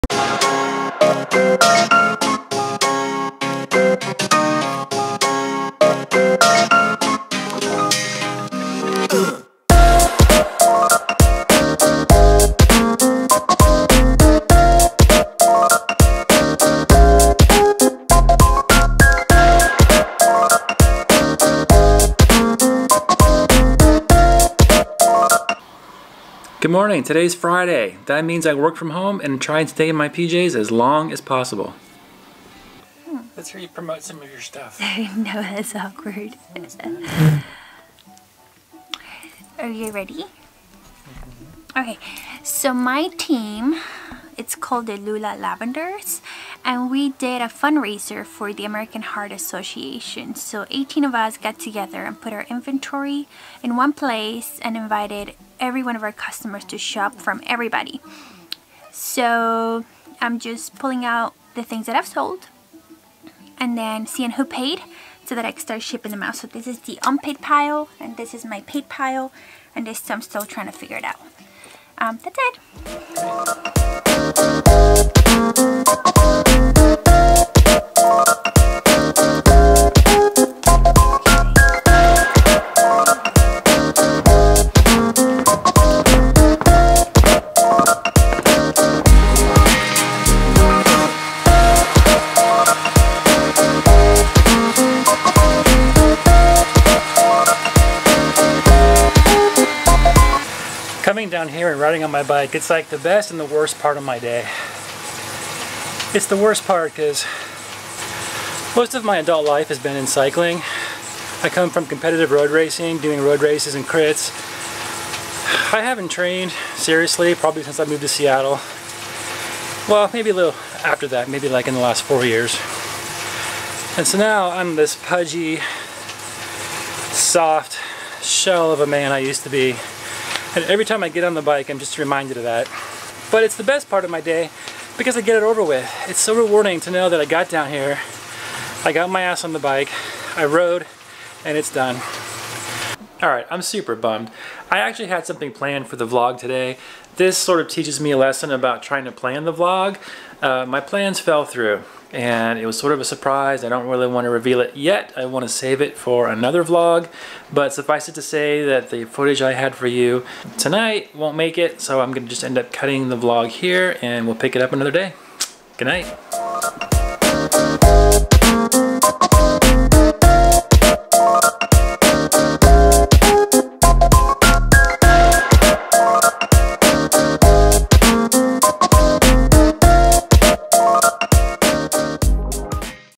Lesson in planning
Today's VLOG didn't quite go as expected but I learned an important lesson in planning a daily VLOG.